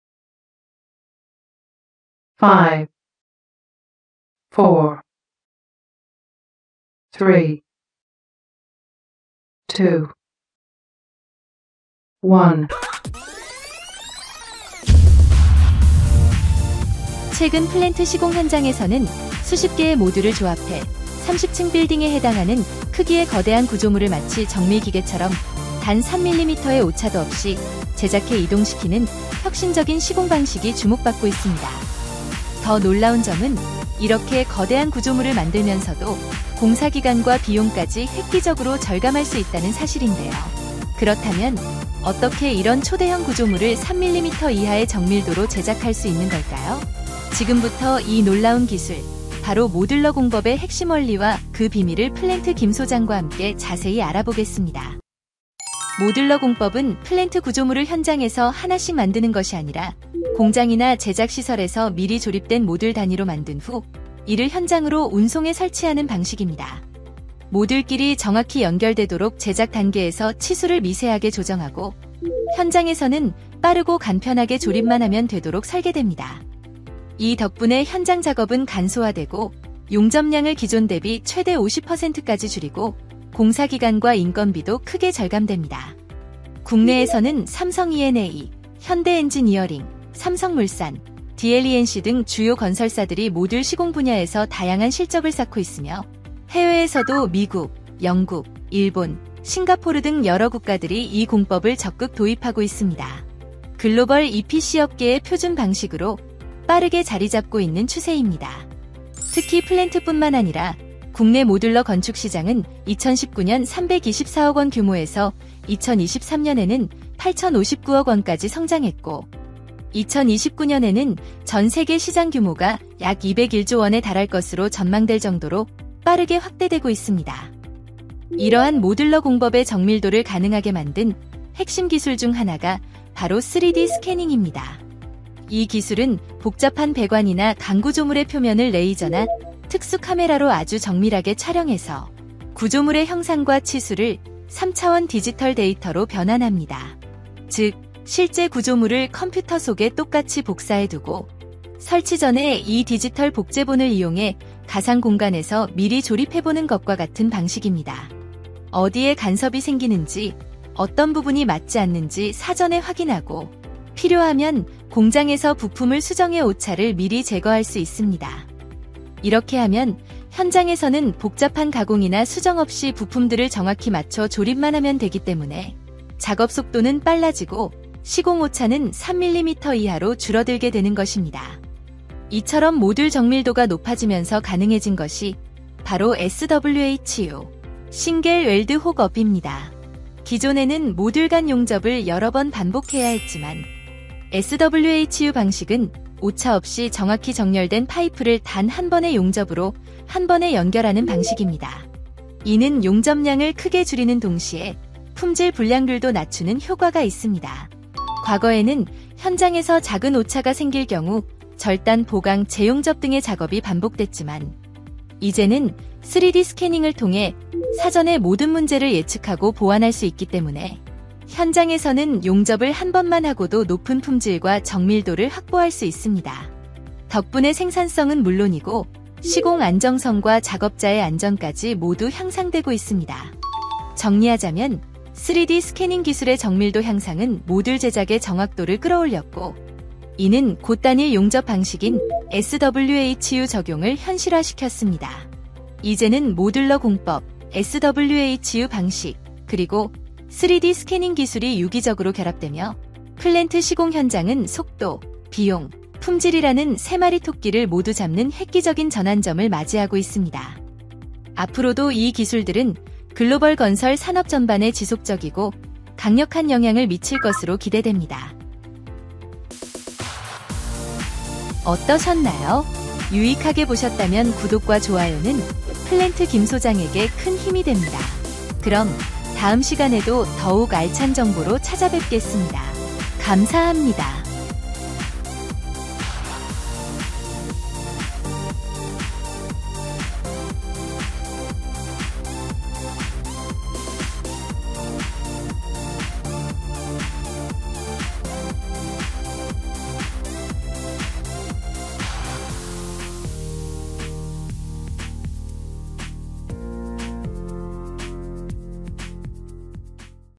📌 용접은 한 번만! 3D 스캐닝 + 모듈러 공법의 혁신 (모듈러 공법 + SWHU + 3D 스캐닝 = 게임 체인저✅)
🔷 30층 구조물을 3mm 오차로?

모듈러 공법: 공장에서 미리 조립해 현장에서 간단하게 설치하는 최신 시공 방식
3D 스캐닝 기술: 구조물 형상과 배관 위치를 정밀하게 디지털화하여 오차 사전 제거
SWHU (Single Weld Hook Up): 단일 용접으로 연결해 품질은 높이고 용접량은 절반 이하로
국내외 도입 현황: 삼성물산, 현대ENG, 삼성E&A 등 국내 대형사뿐 아니라 미국·일본·싱가포르도 적극 도입 중
시장 규모: 국내 모듈러 건축 시장 4년 만에 25배 성장 / 글로벌 201조 원 규모 전망
시공 안정성 향상: 정밀 시공으로 인력 안전성 및 생산성 대폭 향상

💡 왜 주목해야 할까?
모듈러 + 3D 스캐닝 + SWHU는 더 이상 미래 기술이 아닙니다.
이미 현장에서 속도, 비용, 품질을 동시에 잡으며 새로운 표준이 되고 있습니다.
이 영상을 통해 차세대 건설 혁신의 핵심 원리를 쉽고 빠르게 이해해보세요!

플랜트·건축 현장 기술 트렌드를 알고 싶은 분
스마트 건설 및 모듈 시공에 관심 있는 현장 실무자
정밀 시공과 자동화 기술에 주목하는 설계/시공 엔지니어
국내외 EPC 트렌드를 빠르게 파악하고 싶은 관리자

✅ 해시태그 추천